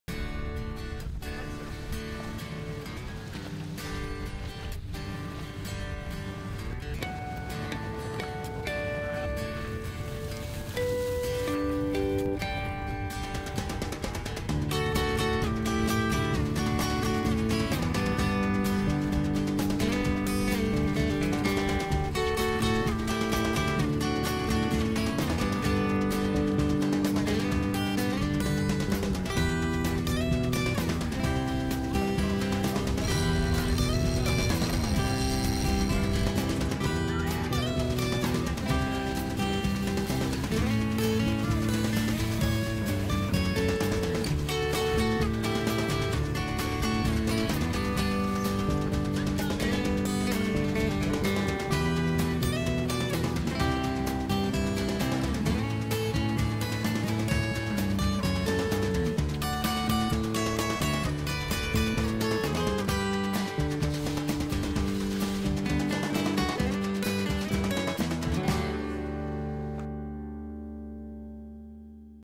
Fishing off the Florida Keys
Check out the fishing action in July 2010, off Islamorada, known as the Sportfishing Capital of the World.